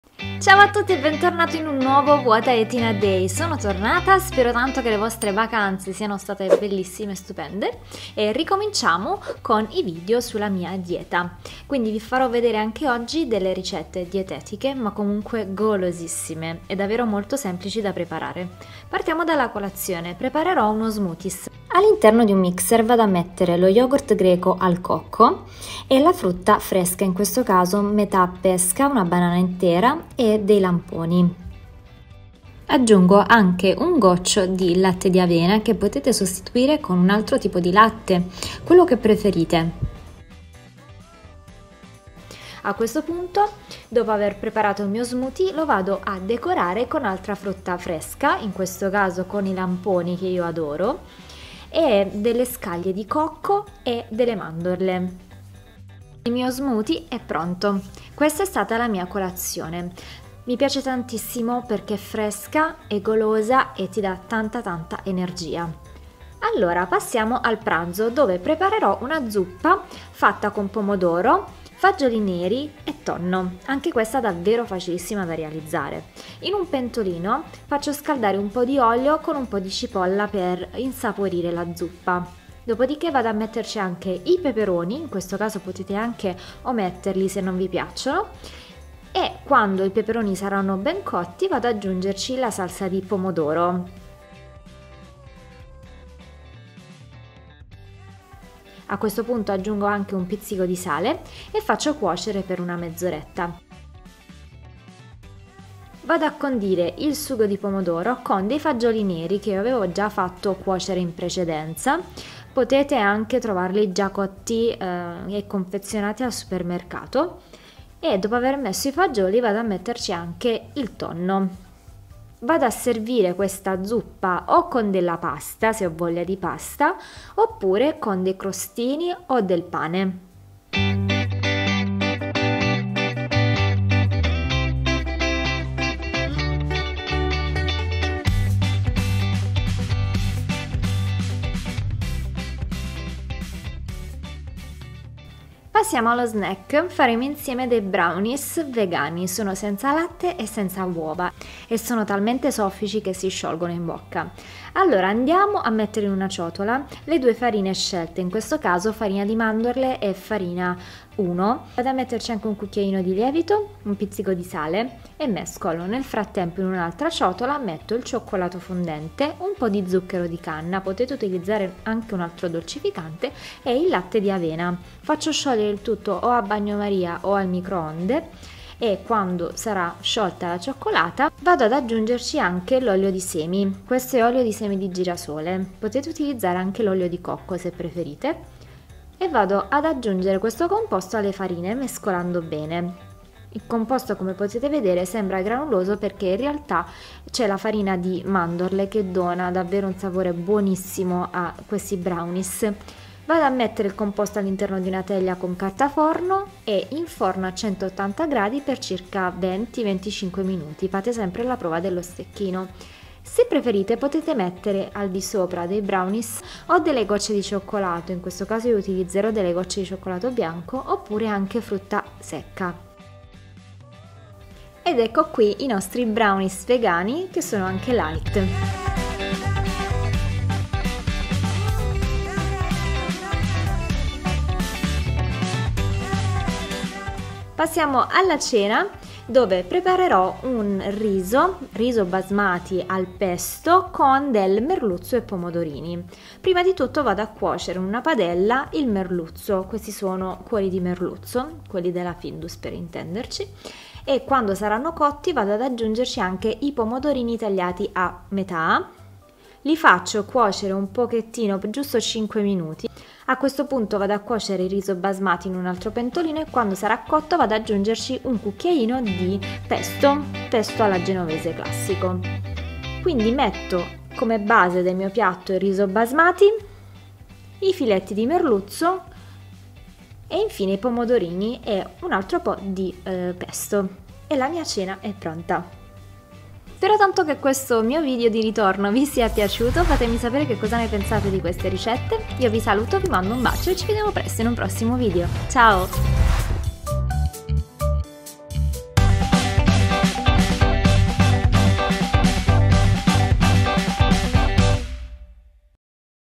COSA MANGIO IN UN GIORNO PER PERDERE PESO -5kg| WHAT I EAT IN A DAY|Jennifer Pignatelli
NUOVO "what I eat in a day" COSA MANGIO IN UN GIORNO di DIETA!
Ricette sane facili LIGHT e gustose
Ricette sane facili LIGHT e gustose perfette per la PROVA COSTUME!
Ho iniziato da poco la dieta, una dieta che prevede poca pasta,almeno una volta a settimana, 2 3 volte pesce e 2 volte carne.
I miei pranzi e le mie cene sono sopratutti PIATTI UNICI composti dal 25% di carboidrati, 25 % di proteine e il 50% di frutta o verdura!
Di solito faccio così:
200 g di verdure
50 g di carboidrati
50 g di proteine
Vario anche in base alle mie esigenze..
Tutto questo in accoppiata a movimento e attivitá fisica mi ha permesso di perdere 5 kg in un mese!

➡️ SOCIAL ⬅️
►INSTAGRAM shorturl.at/iILRZ
►FACEBOOK shorturl.at/azIW7